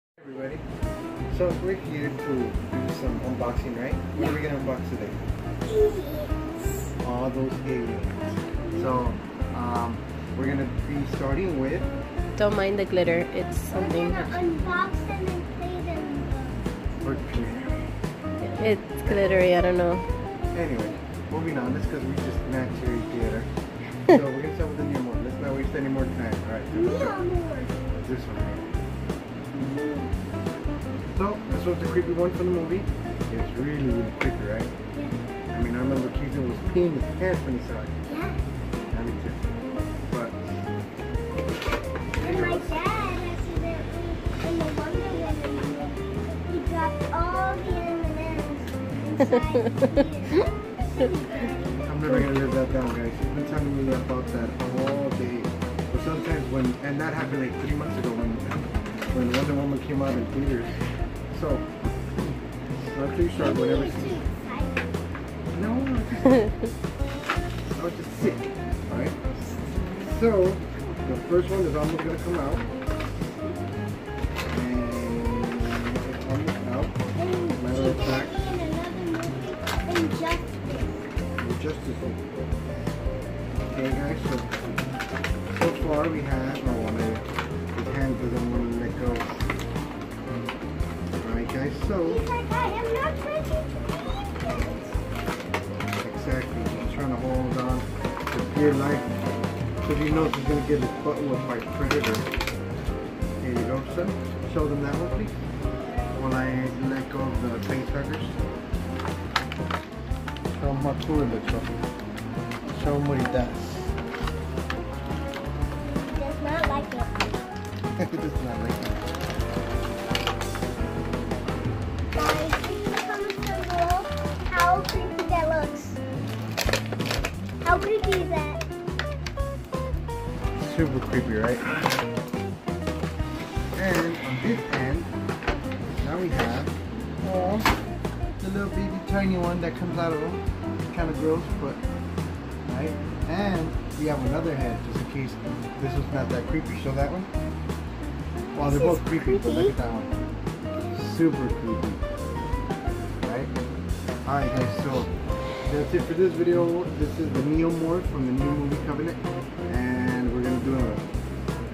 Unboxing Alien Covenant Neomorph with Kassen and Papi
Watch us unbox an Alien Covenant Neomorph action figure which is super creepy and can give you nightmares just by looking at its scary face! Kassen loves these creepy action figures and so do I. Hope you guys enjoy our video.